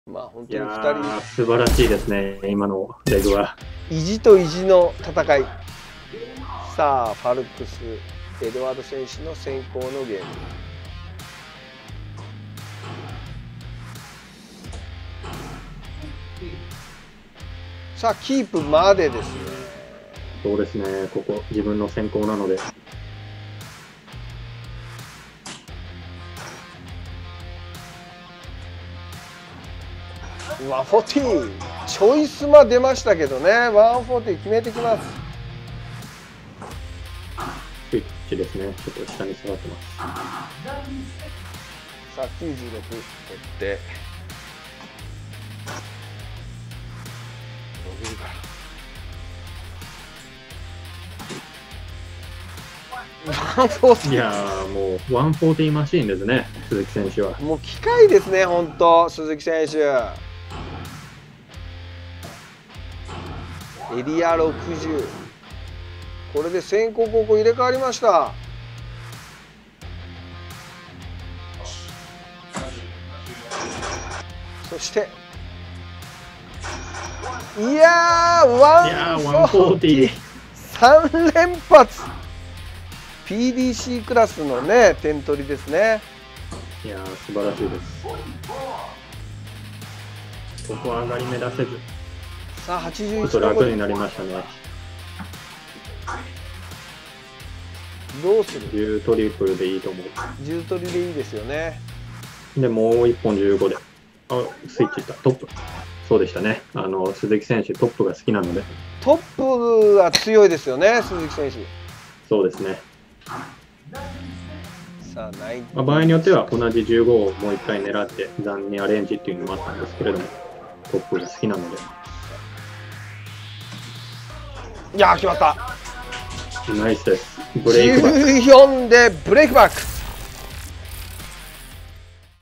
『140マシン』スズキ トオルvsF.S.エドワード【FIDO ONLINE CUP 2021 TOUR 第5戦】
FIDO ONLINE CUP 2021 TOUR  第5戦（2021.7.4）

【BEST32】
スズキ トオルvsF.S.エドワード

～内容～
FIDOdarts のオンライン対戦を使用し
各エリアのカウントアップ点数上位者による
オンライントーナメント